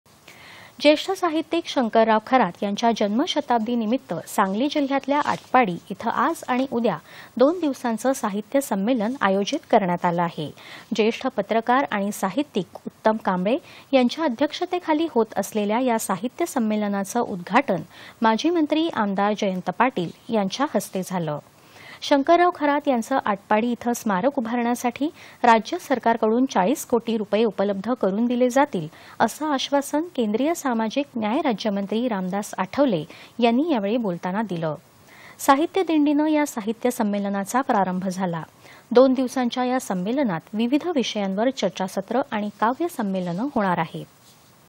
सांगली जिल्ह्यातल्या आटपाडी इथं आज आणि उद्या दोन दिवसांचं साहित्य संमेलन
दि. १०.०७.२०२२ :
      ज्येष्ठ साहित्यिक शंकरराव खरात यांच्या जन्म शताब्दी निमित्त सांगली जिल्ह्यातल्या आटपाडी इथं आज आणि उद्या दोन दिवसांचं साहित्य संमेलन आयोजित करण्यात आलं आहे.
     ज्येष्ठ पत्रकार आणि साहित्यिक उत्तम कांबळे यांच्या अध्यक्षतेखाली होत असलेल्या या साहित्य संमेलनाचं उद्घाटन माजी मंत्री आमदार जयंत पाटील यांच्या हस्ते झालं.
      शंकरराव खरात यांचं आटपाडी इथं स्मारक उभारण्यासाठी राज्य सरकारकडून ४० कोटी रुपये उपलब्ध करुन दिले जातील, असं आश्वासन केंद्रीय सामाजिक न्याय राज्यमंत्री रामदास आठवले यांनी यावेळी बोलताना दिलं.
     साहित्य दिंडीनं या साहित्य संमेलनाचा प्रारंभ झाला. दोन दिवसांच्या या संमेलनात विविध विषयांवर चर्चासत्र आणि काव्य संमेलन होणार आहेत.